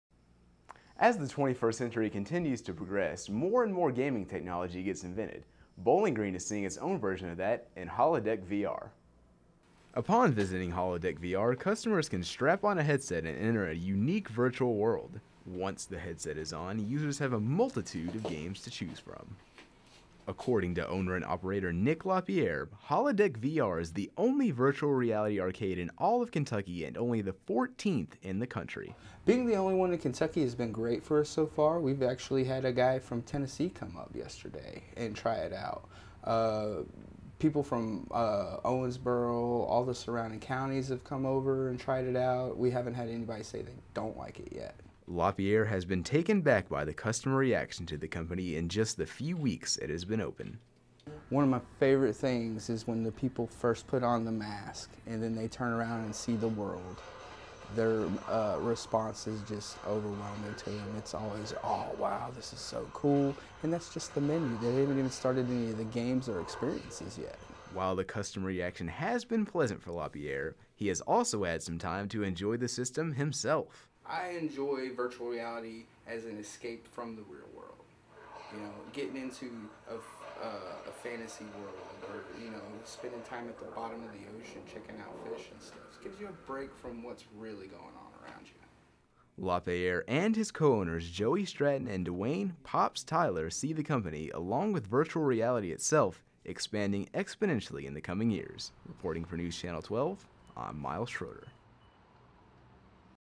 HoloDeck VR - Bowling Green
The first Virtual Reality Arcade in Kentucky set in sights in Bowling Green